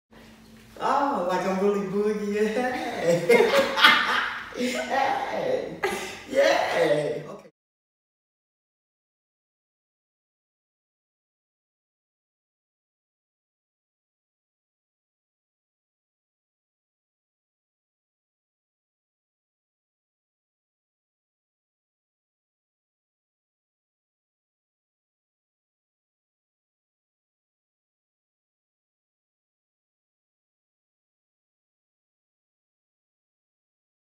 Motivated Amputee Does a Joyful Dance While Learning to Balance on Prosthetic Leg
Our awesome patient, Sonya, is a right below knee amputee who is able to instantly light up a room when she enters.

She is motivated to ambulate with a prosthesis because she has a grandchild and finds it hard to perform many activities from a wheelchair.

Sonya is wearing a Bulldog Tools Lock and REVOlimb Boa Fit System, a WillowWood Alpha Hybrid Select Locking liner, and a Freedom Innovations Agilix foot.